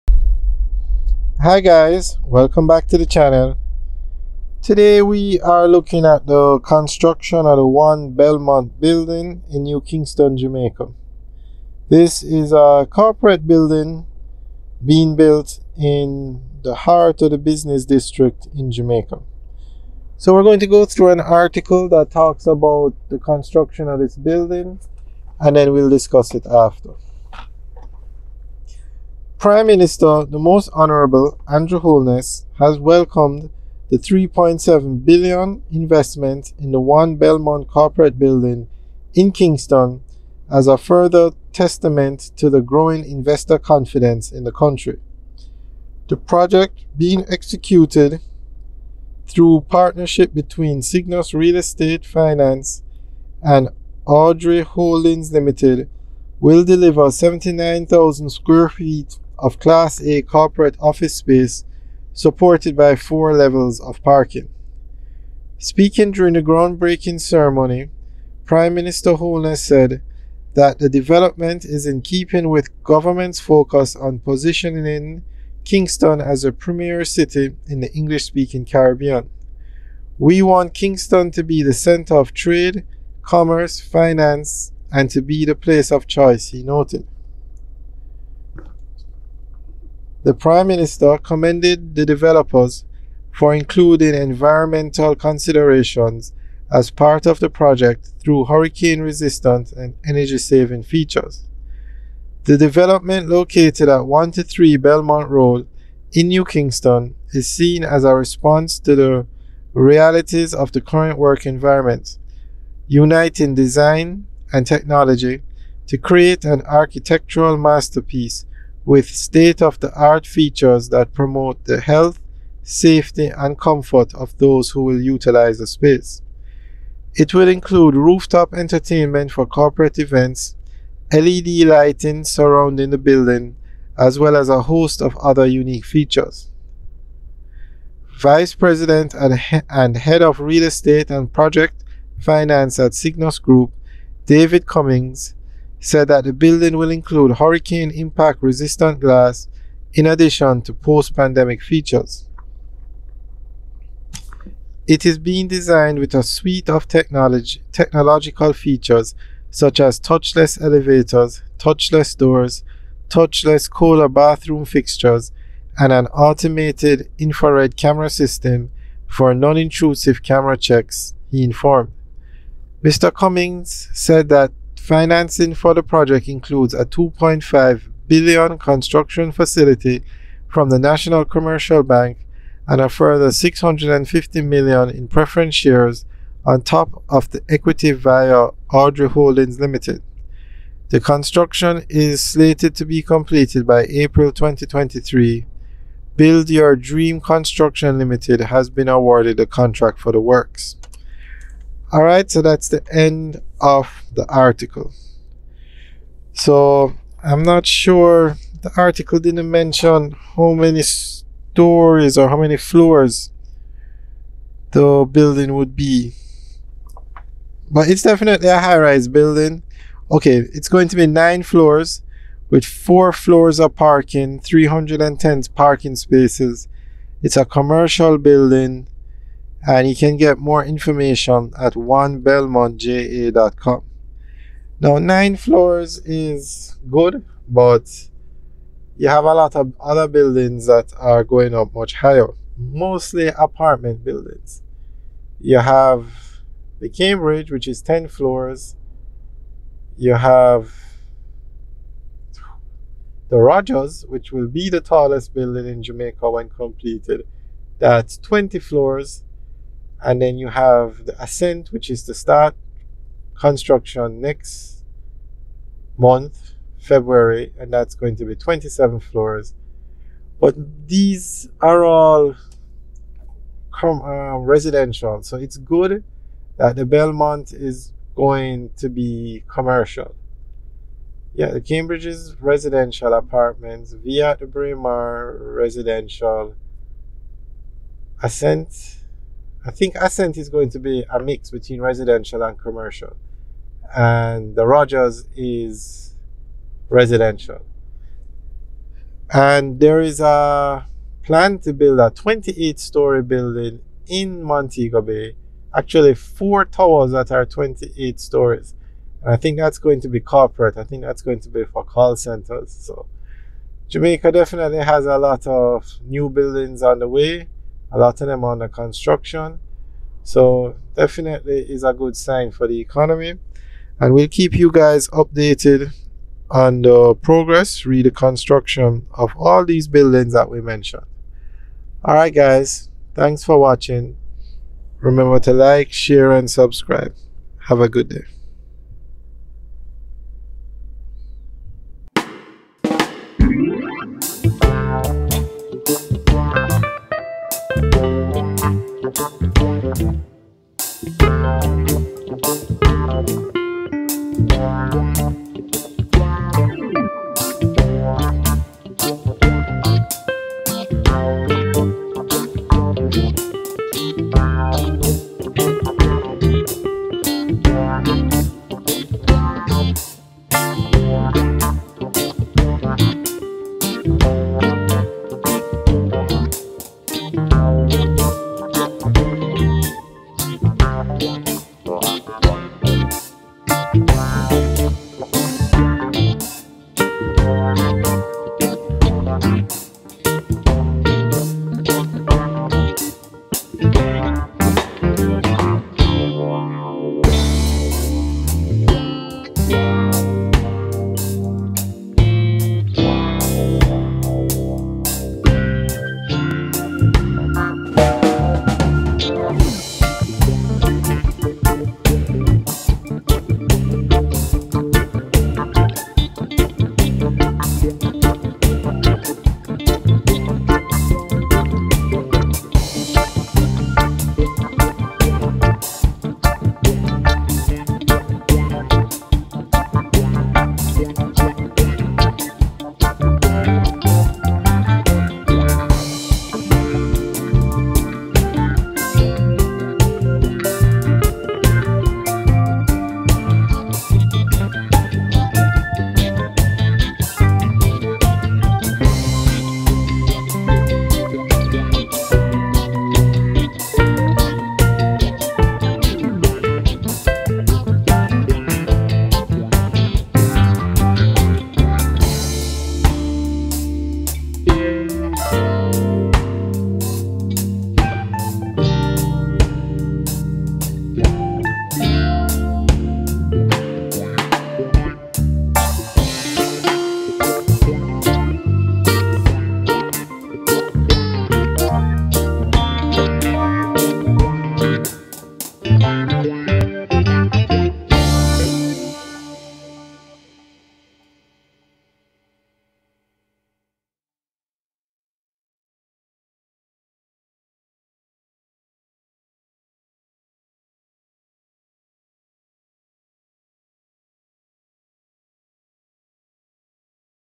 Construction One Belmont Building Jamaica
A narrated driving tour One Belmont Building New Kingston Jamaica